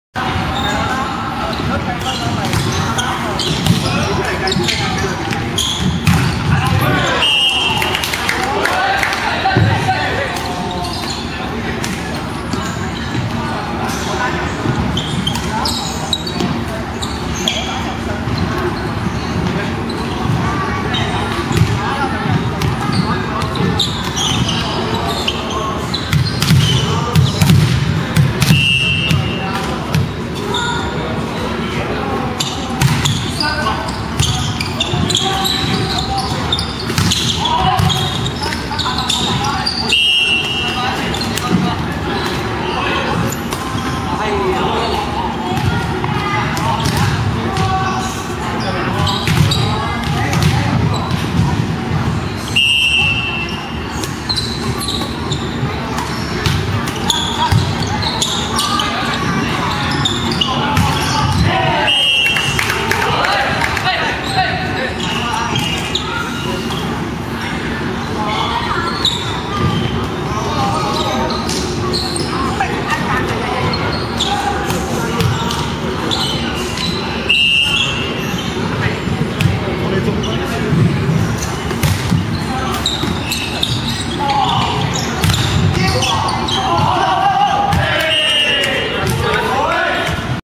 體育精神的展現：男子排球比賽 20130120
2012年度第三十一屆全港公開馬拉松排球賽 (公開組)
比賽日期：2013年1月20日(星期日)
比賽地點：石硤尾公園體育館
比賽時間：下午3時31分
仁青 B VS APOPTSIS